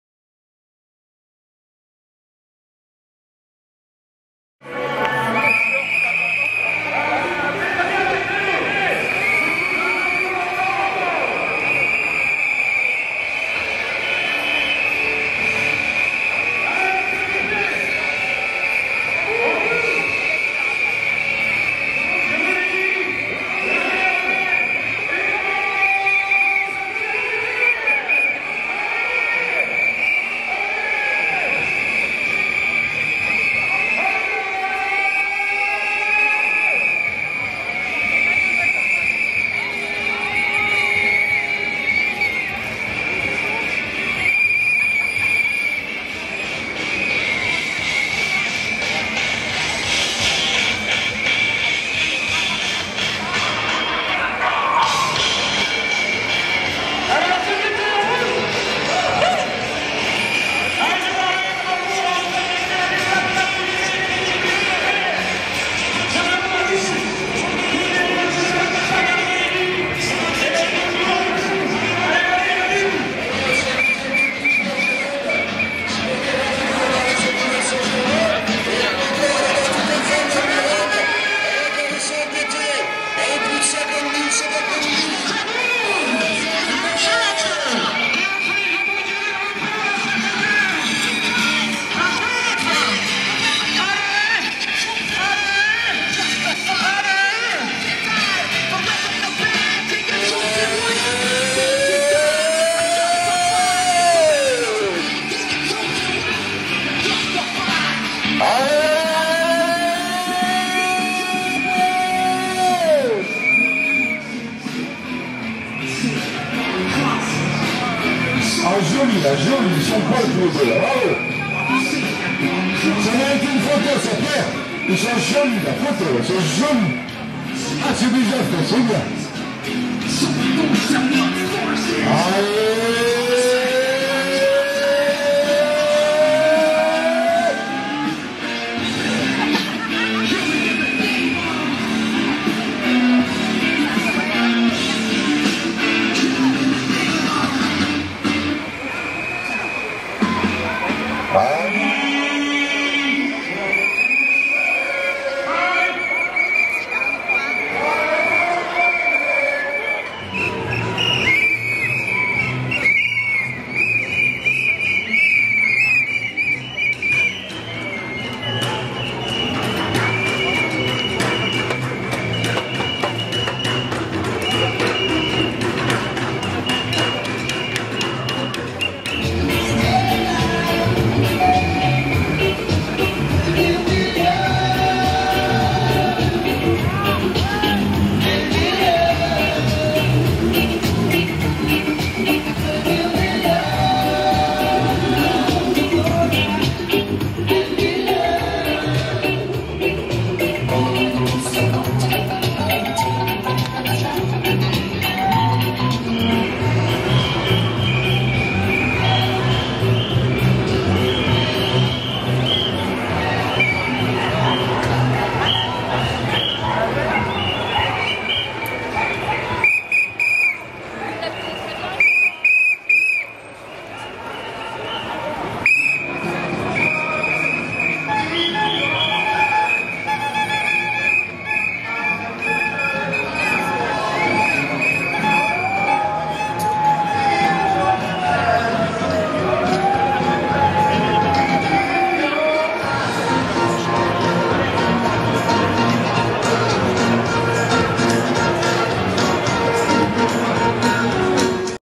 paris MANIFESTATION 12 OCTOBRE 2010
paris MANIFESTATION 12 OCTOBRE 2010, contre la reforme des retraites...